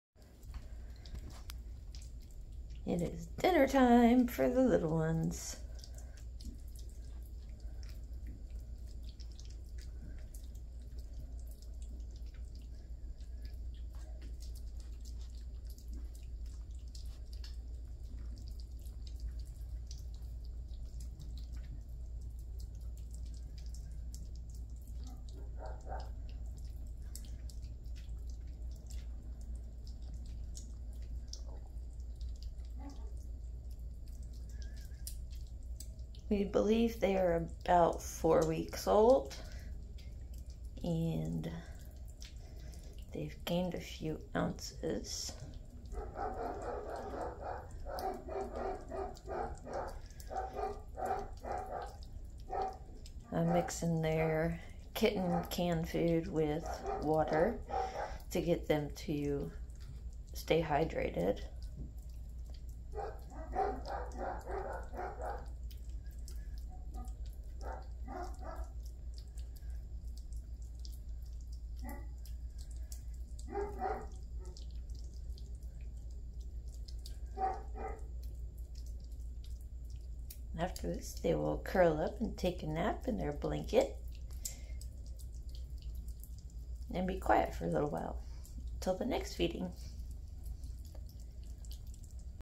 Dinner time for the three 07/07/23
The little orange and white male, we named Tobi, is being fostered by a volunteer. His sisters are eating like little chunks. 😁